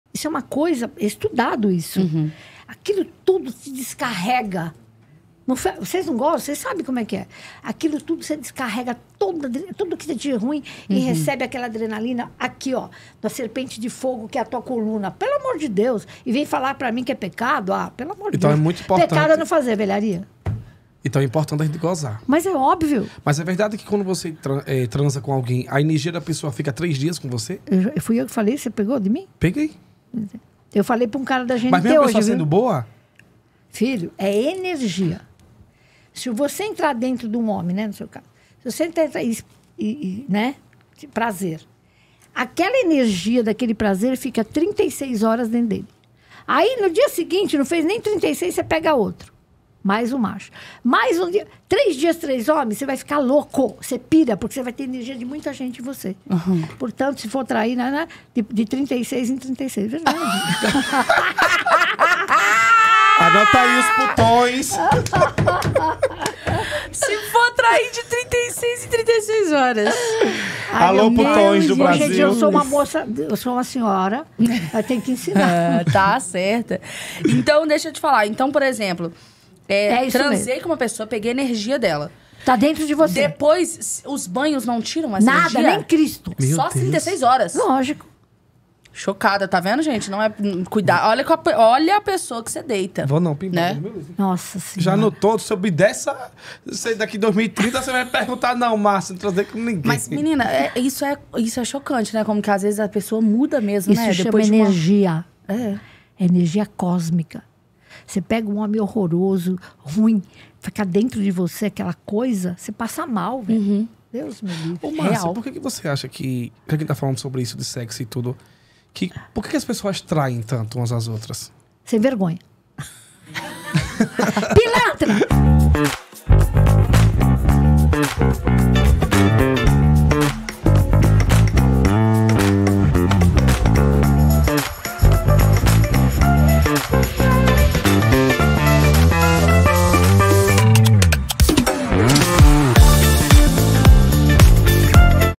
VEJO O LEWIS HAMILTON MORRER!
Esse é o meu, o seu, o nosso PodCats 😻❤️

Hoje vamos receber a icônica, Márcia Sensitiva! Quais serão as próximas previsões dela? Será que ela acerta quem vai ganhar a Copa? Saberemos em primeira mão aqui no Podcats! Não percam esse papo babadeiro, do jeitinho que só ela sabe! 😻❤️

✅ Instagram Oficial

CONVIDADA:

ANFITRIÃS
👩🏽Camila Loures: @camilaloures
👨🏻Lucas: @lucasguimaraes